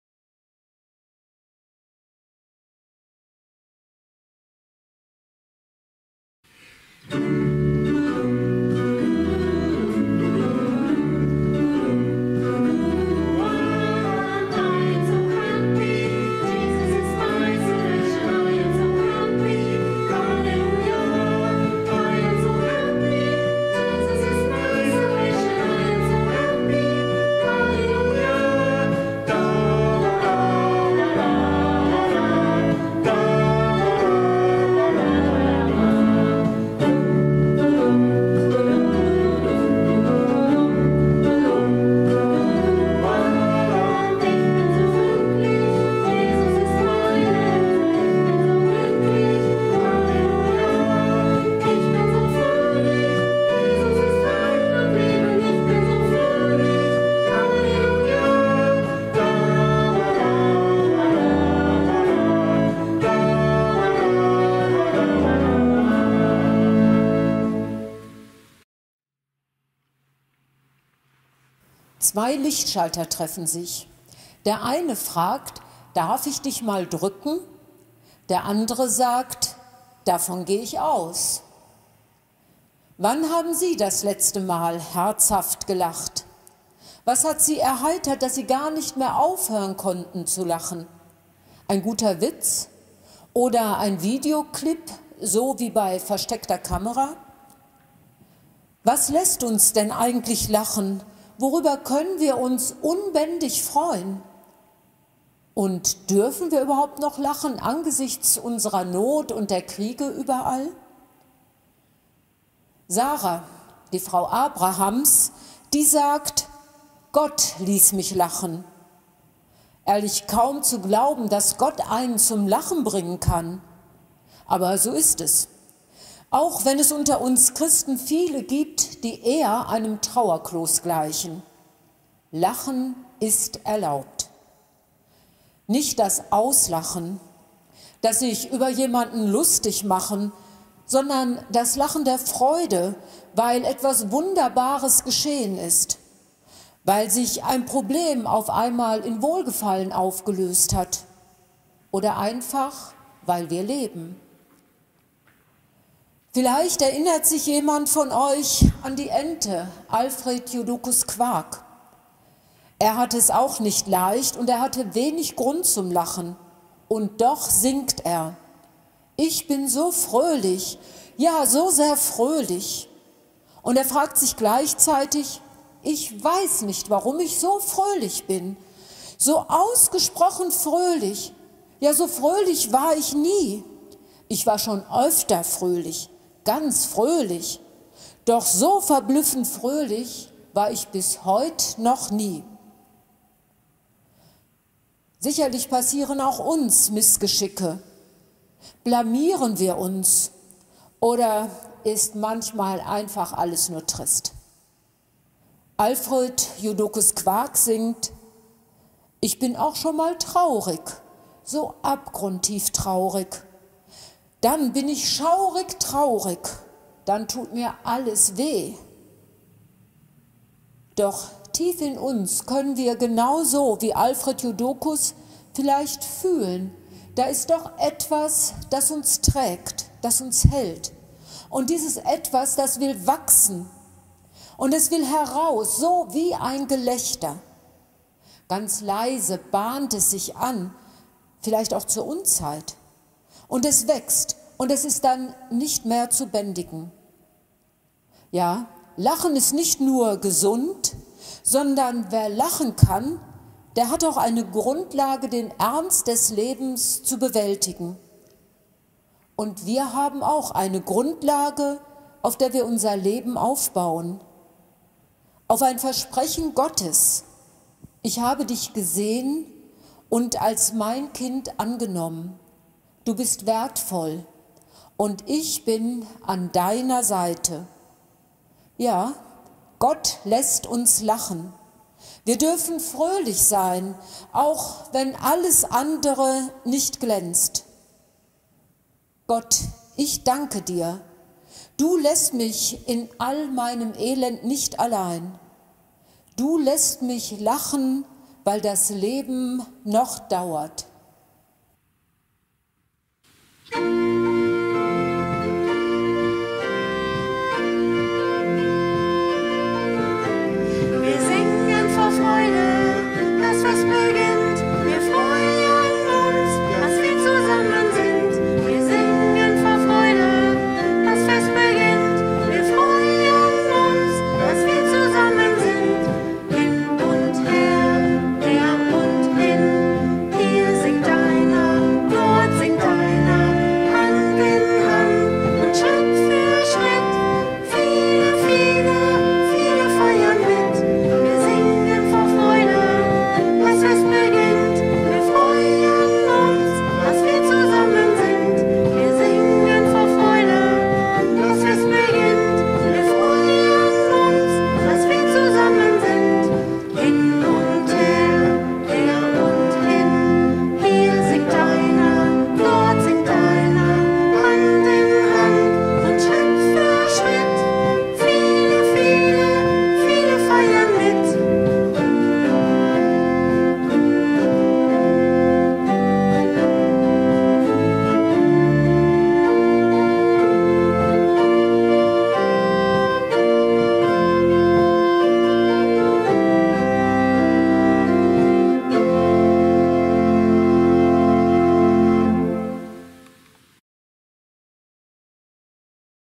Februar 2023 - Impuls der Evang. Kirchengemeinde DO-Scharnhorst
Impuls im Monat Februar 2023 aus der evangelischen Auferstehungskirche in Dortmund-Scharnhorst mit Pfarrerin Ruth Niehaus. "Lachen ist erlaubt"